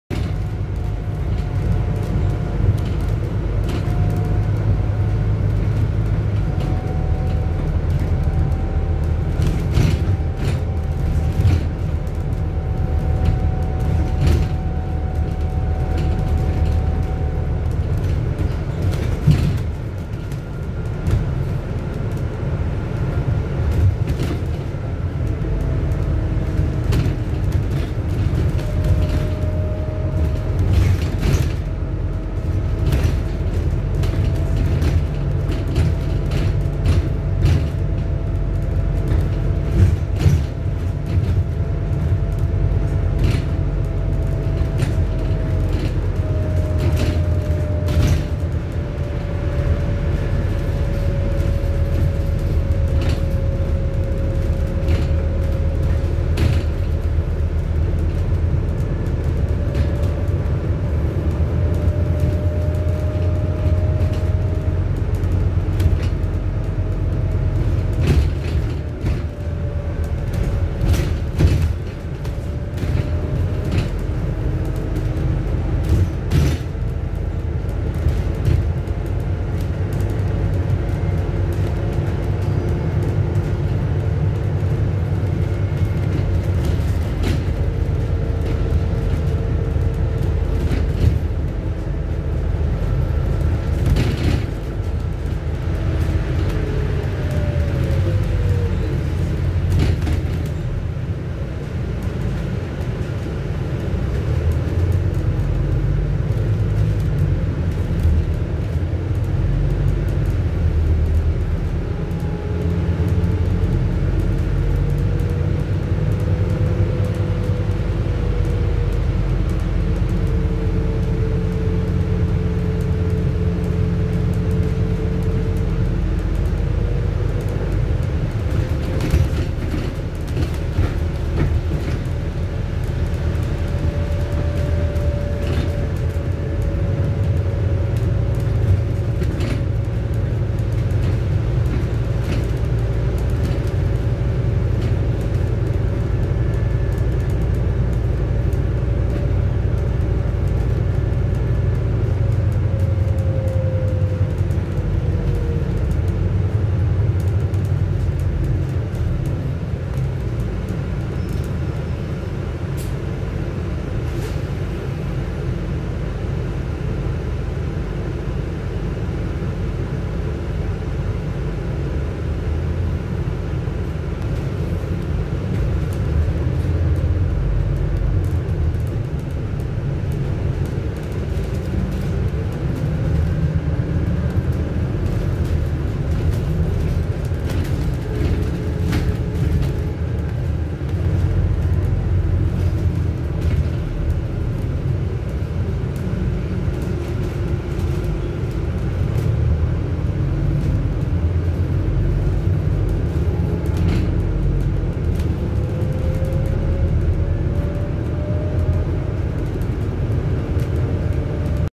Stagecoach South West 15317 YN67 YKA
Here we see a journey on Stagecoach South West Scania Enviro 400 MMC 15317 YN67 YKA seen on route 12 to Brixham.